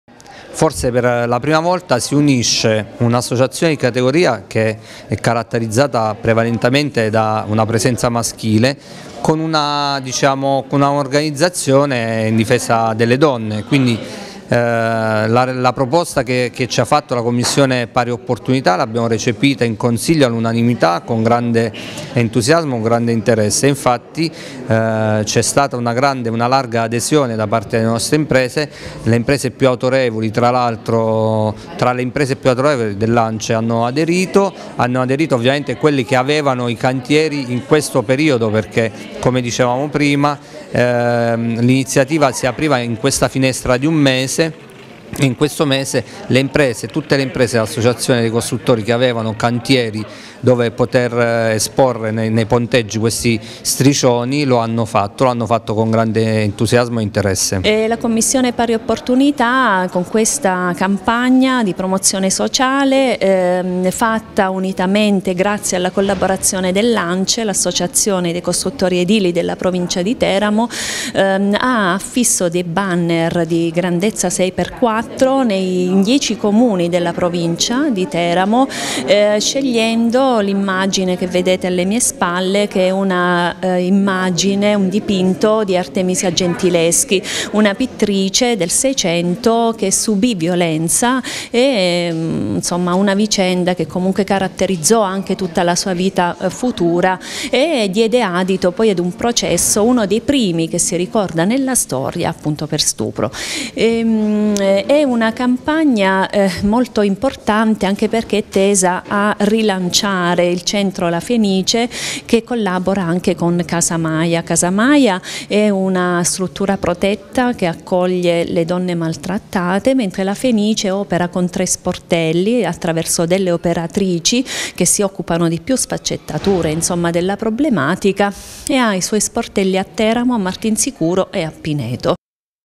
Ance e Cpo contro la violenza sulle donne
“Mai più in silenzio” è una campagna di comunicazione sociale ideata dalla Commissione pari opportunità della Provincia di Teramo. Dieci le imprese edili, raccolte sotto l’insegna dell’Associazione Costruttori edili, che hanno già installato banner giganti sulle facciate dei cantieri e su alcuni immobili pubblici in dieci cittadine della provincia